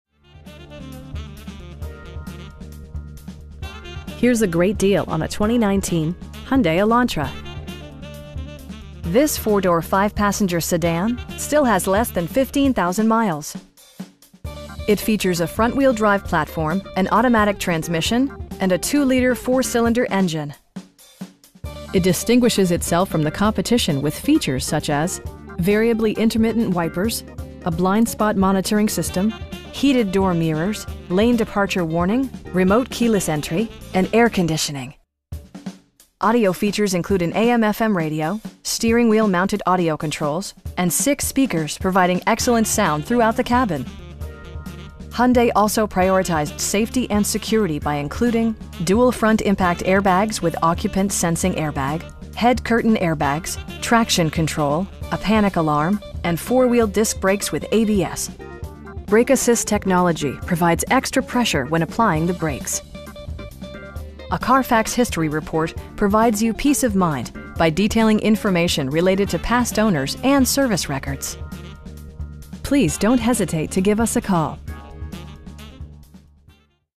2019 Hyundai Elantra SEL in Wichita Falls, TX 76301
Patterson Honda
319 Central Freeway E

Here's a great deal on a 2019 Hyundai Elantra. This 4 door, 5 passenger sedan still has less than 15,000 miles! It features a front-wheel-drive platform, an automatic transmission, and a 2 liter 4 cylinder engine. It distinguishes itself from the competition with features such as: variably intermittent wipers, a blind spot monitoring system, heated door mirrors, lane departure warning, remote keyless entry, an overhead console, and air conditioning. Audio features include an AM/FM radio, steering wheel mounted audio controls, and 6 speakers, providing excellent sound throughout the cabin. Hyundai also prioritized safety and security by including: dual front impact airbags with occupant sensing airbag, head curtain airbags, traction control, a panic alarm, and 4 wheel disc brakes with ABS. Brake assist technology provides extra pressure when applying the brakes. A Carfax history report provides you peace of mind by detailing information related to past owners and service records. You will have a pleasant shopping experience that is fun, informative, and never high pressured. Please don't hesitate to give us a call.